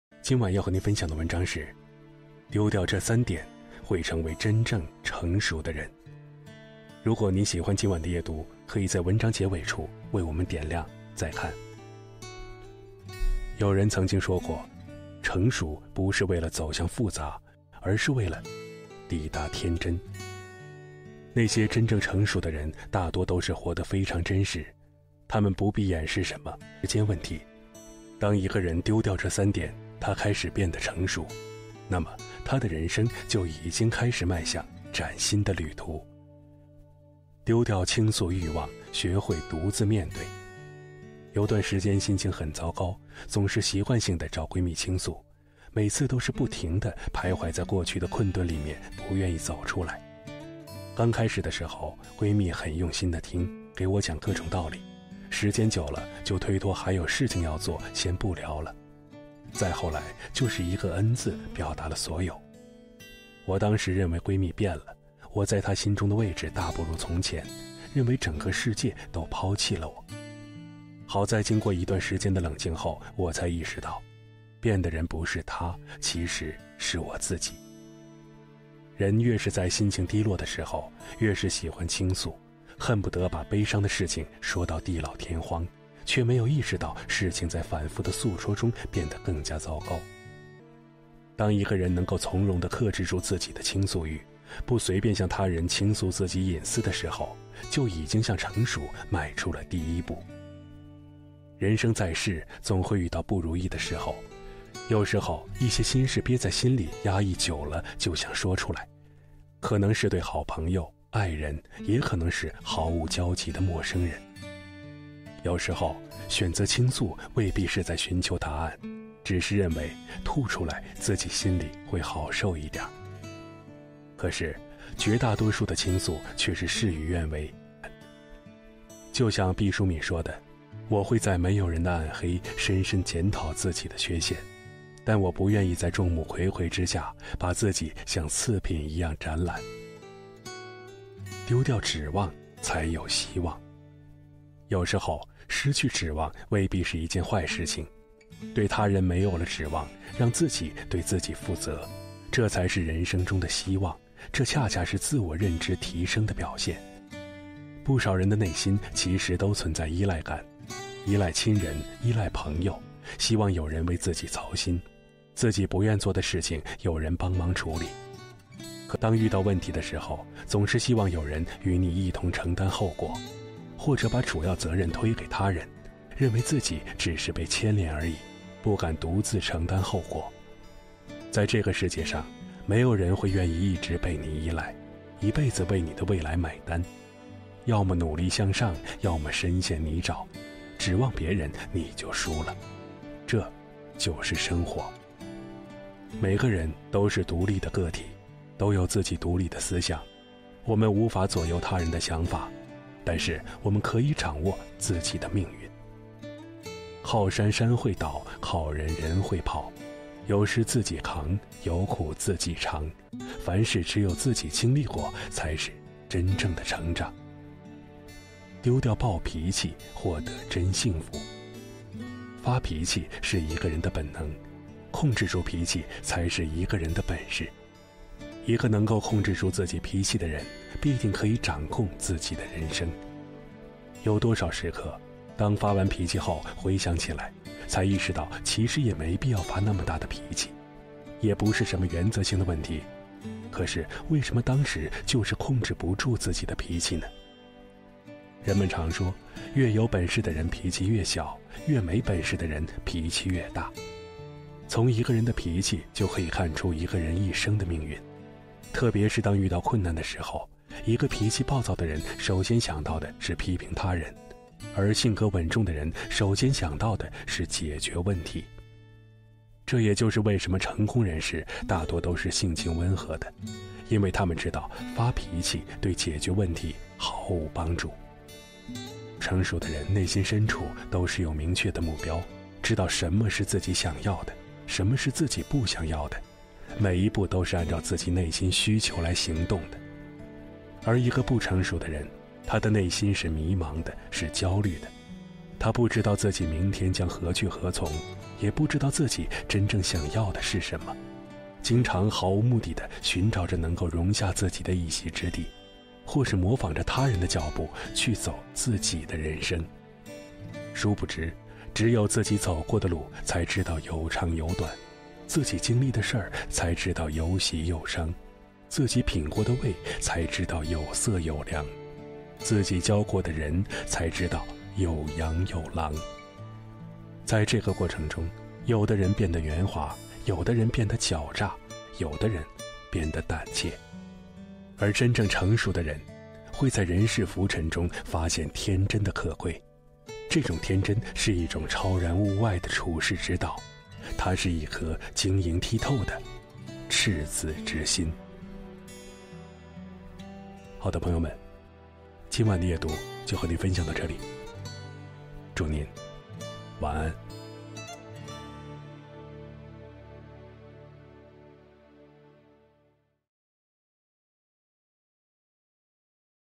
夜读 丢掉这三点，会成为真正成熟的人#情感语录【深夜读书】（十四）
夜读  丢掉这三点，会成为真正成熟的人#情感语录【深夜读书】成熟不是为了走向复杂，而是为了抵达天真。